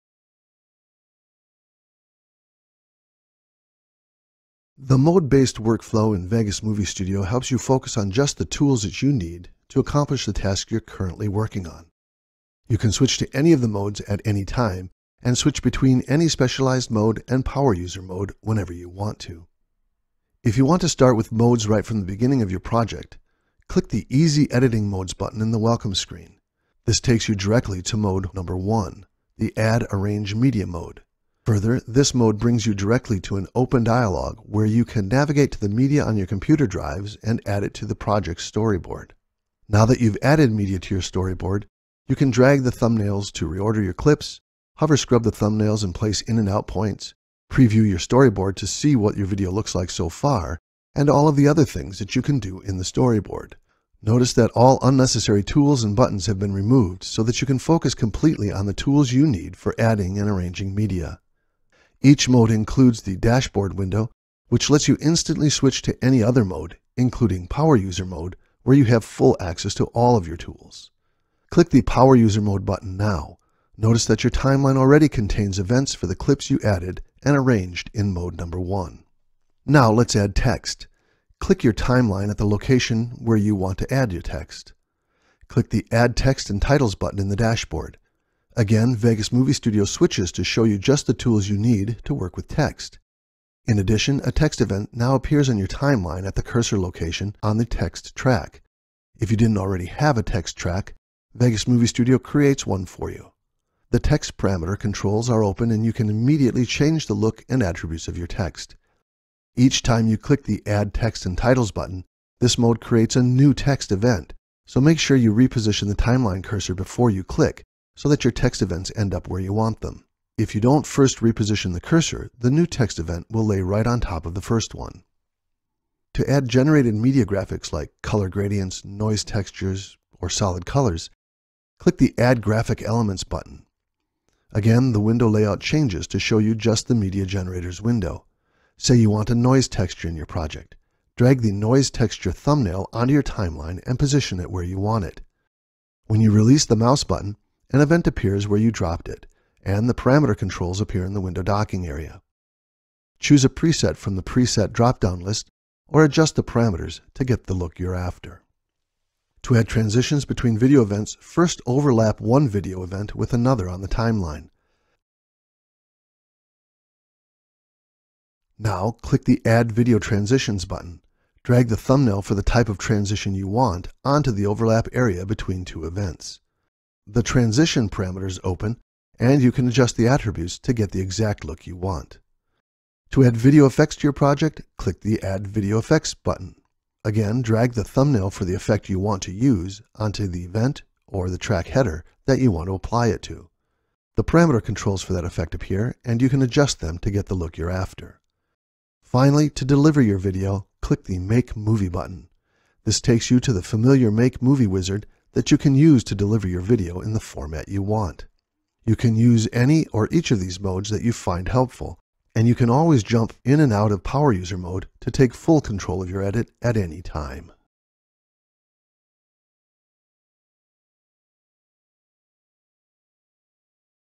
VEGAS Movie Studio 16 - Optional Mode-Based Workflows Tutorial
This tutorial video shows how to edit your videos and add titles, transitions, effects, and more with the new mode-based workflows.